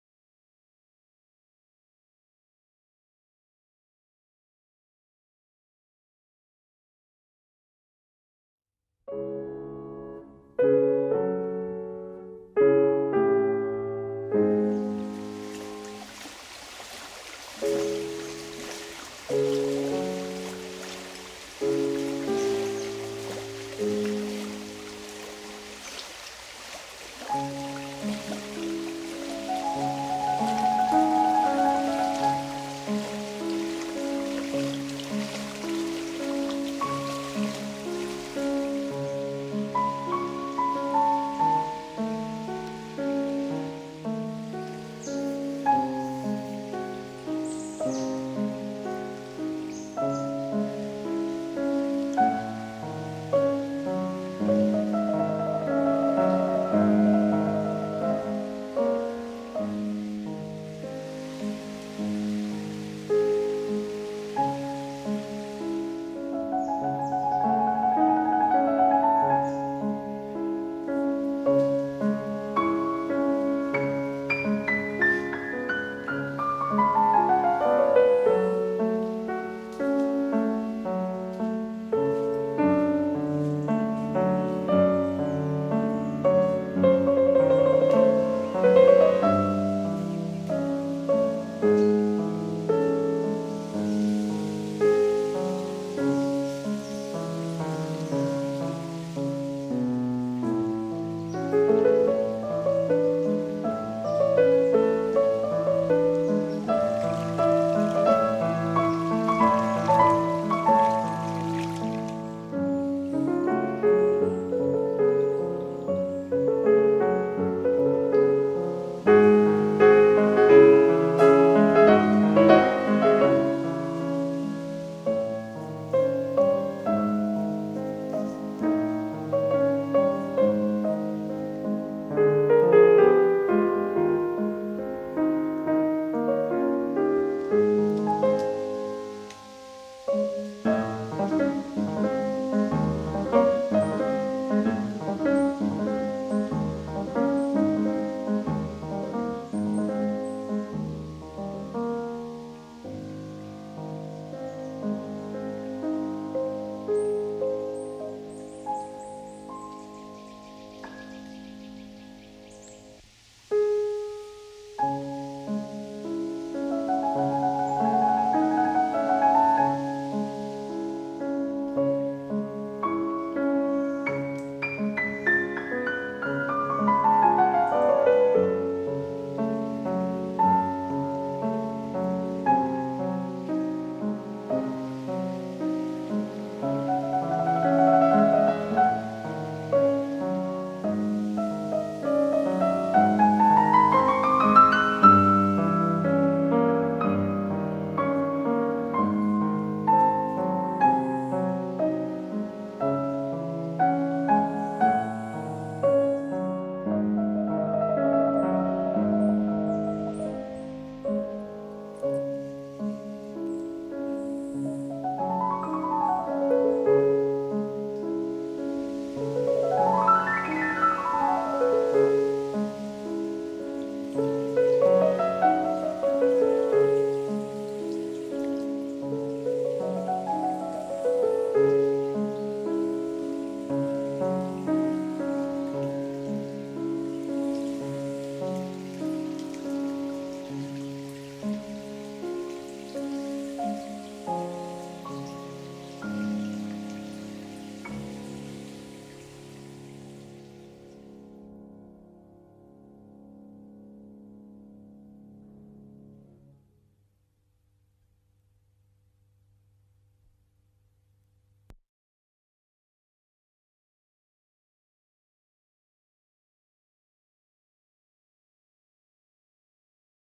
Radunia (Kaszuby)
Kaszuby. Okolice Żukowa. Rzeka Radunia płynie tutaj malowniczym jarem...
Poland. Kashube(?). Ghyll (gulch) of Radunia river